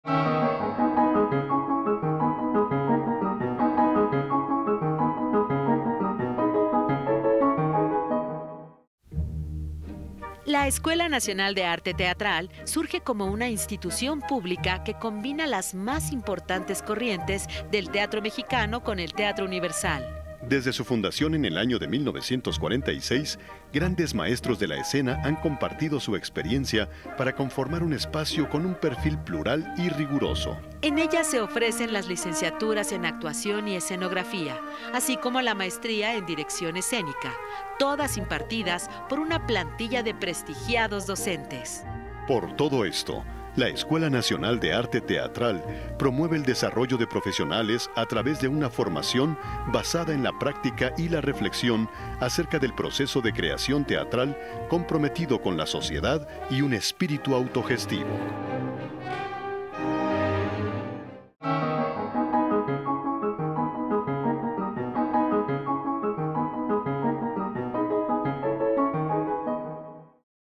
Escuela Nacional de Arte Teatral
Desde 1946, la Escuela Nacional de Arte Teatral abre sus puertas para que puedas cursar las licenciaturas de actuación y escenografía así como la maestría en dirección escénica. Todas impartidas por importantes docentes que te brindarán la preparación que necesitas. ¡Ven a visitarla para que descubras todo lo que la institución te puede ofrecer!